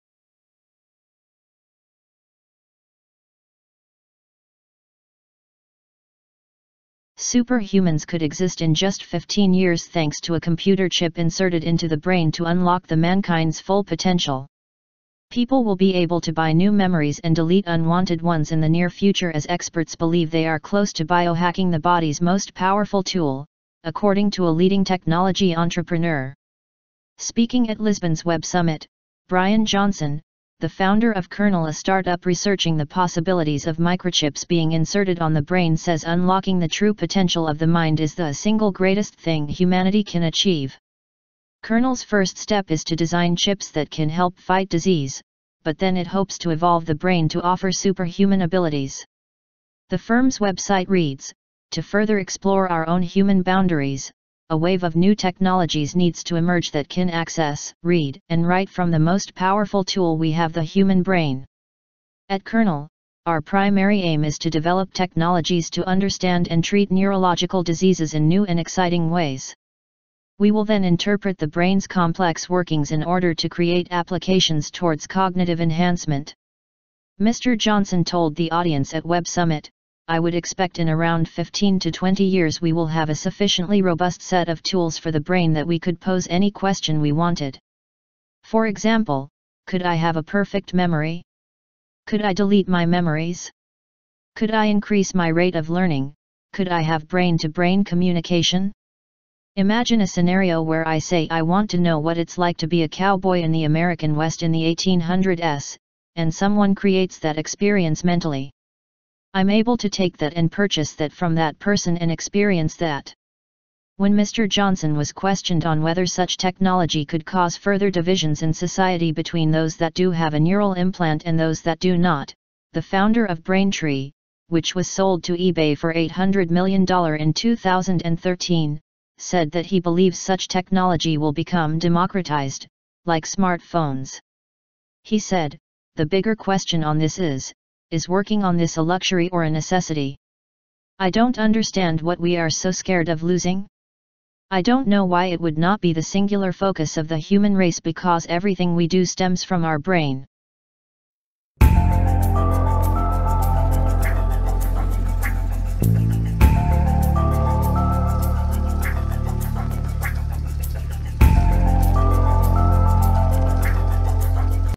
SUPER-HUMANS: Chips inserted in brains will give us MIND BLOWING abilities
SUPER HUMANS could exist in just 15 years thanks to a computer chip inserted into the brain to unlock the mankind’s full potential.